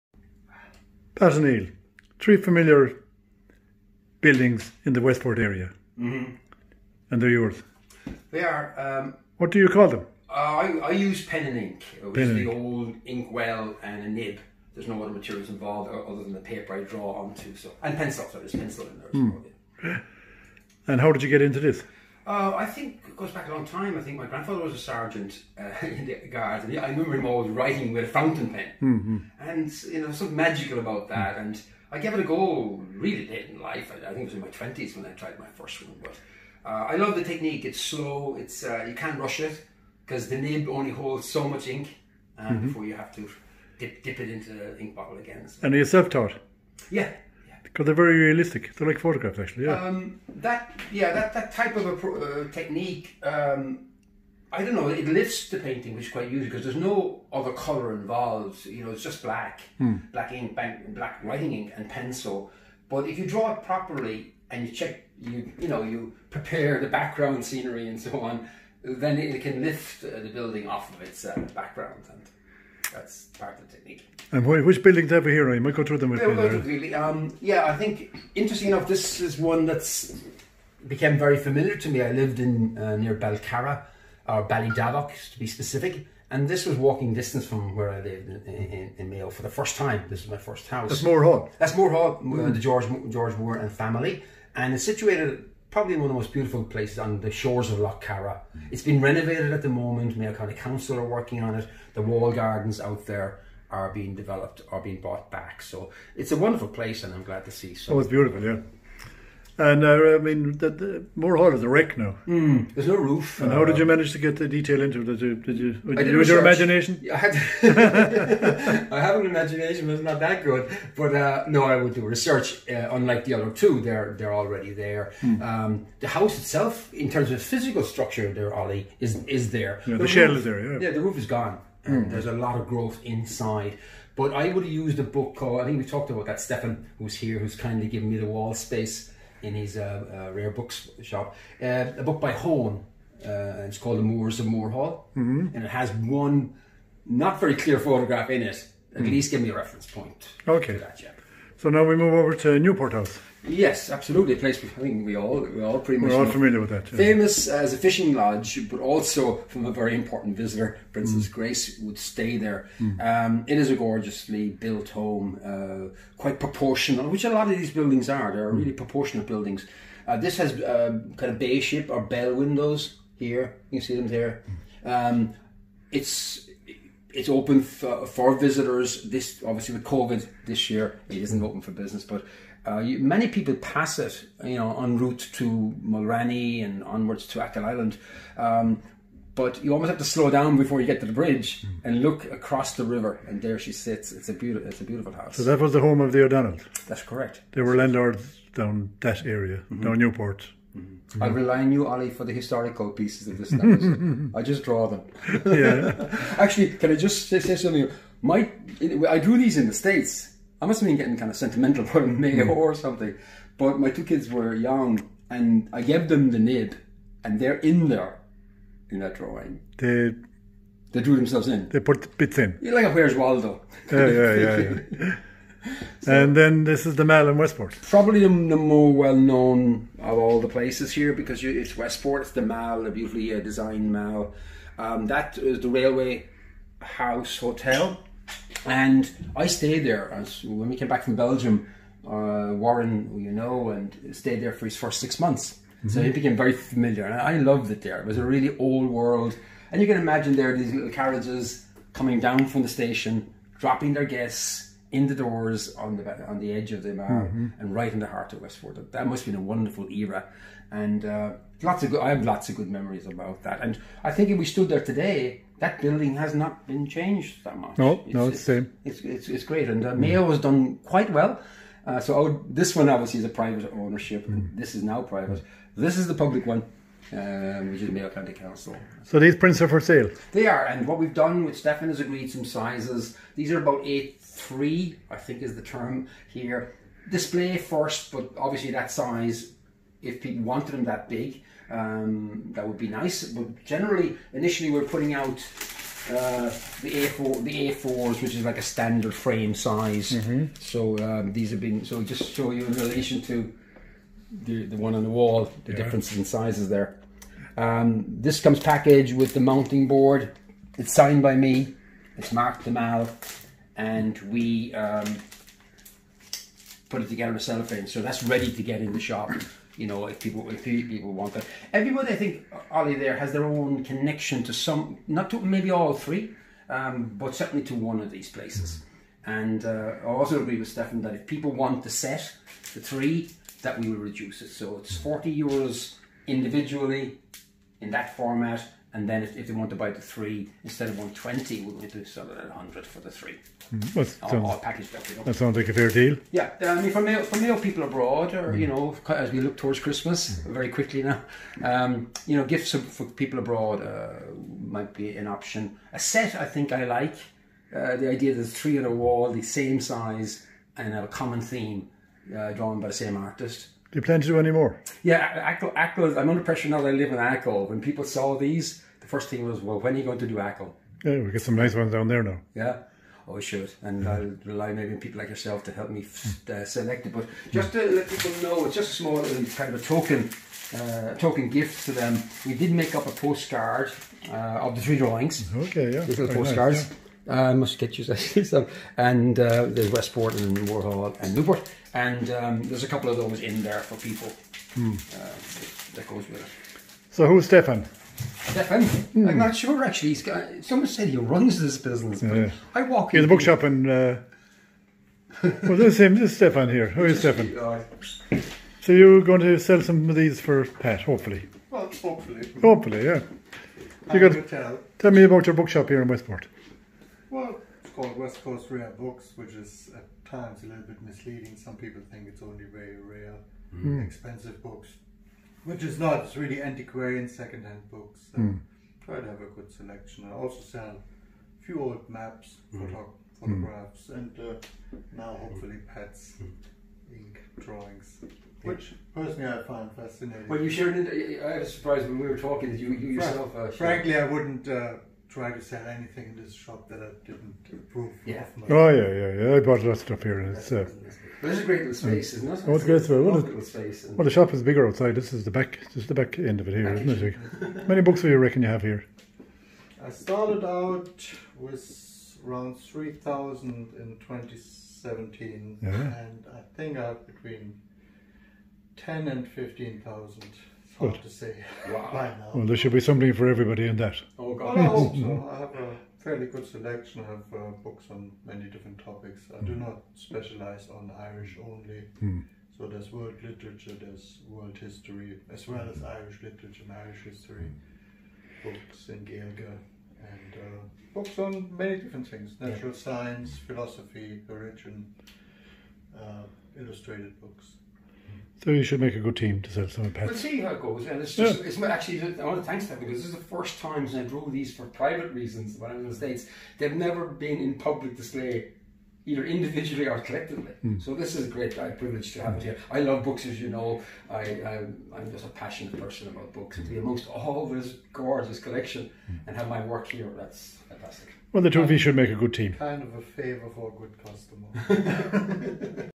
Pat O Neill: Artist. 17.7.2020.
Pat O Neill talks about his art in Westcoast Rare Books, Westport, Ireland. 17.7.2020